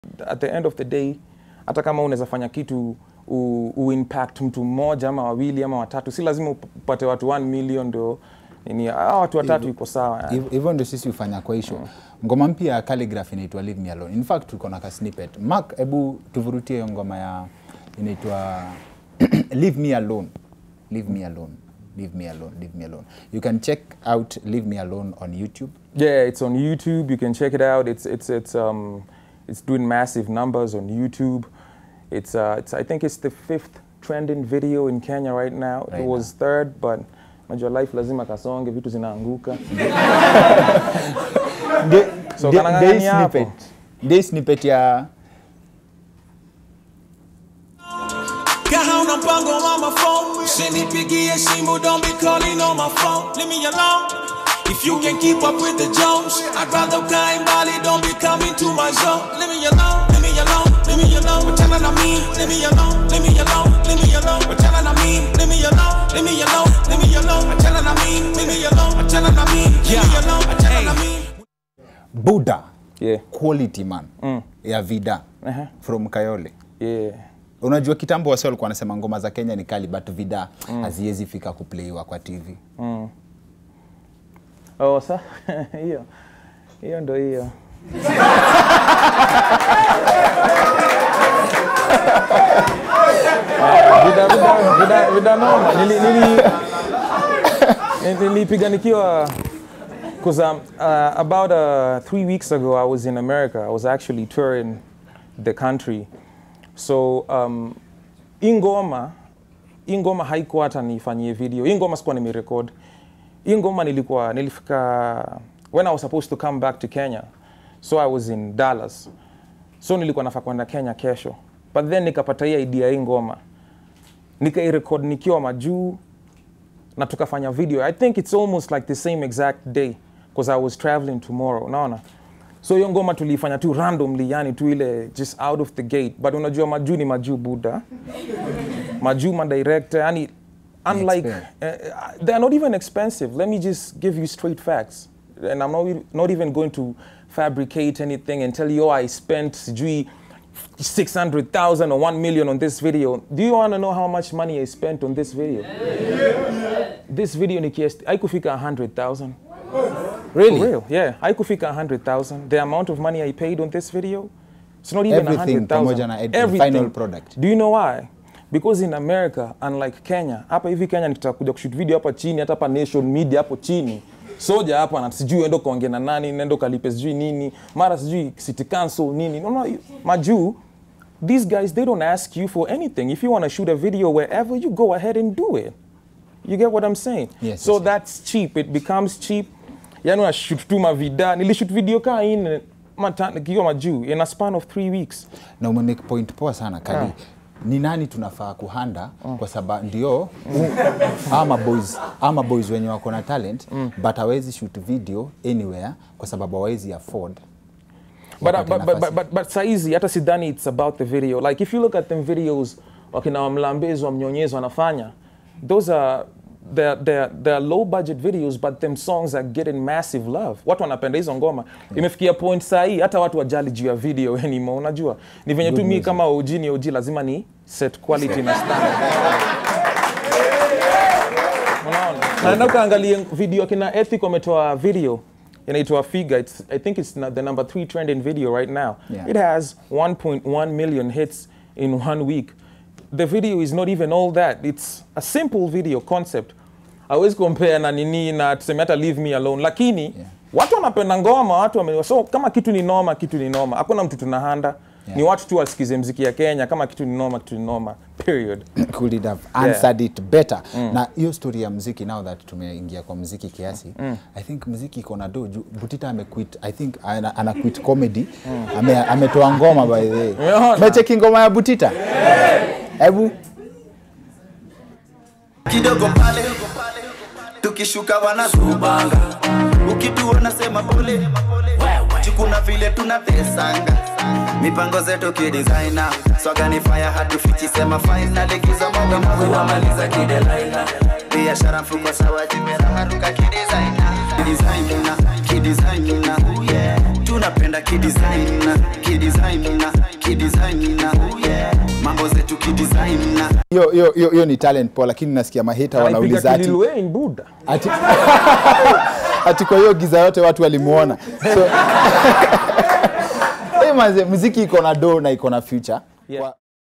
Khaligraph reveals how much he spent on his latest music video abroad
What's the real cost of a music video outside Kenya? Khaligraph Jones breaks it down.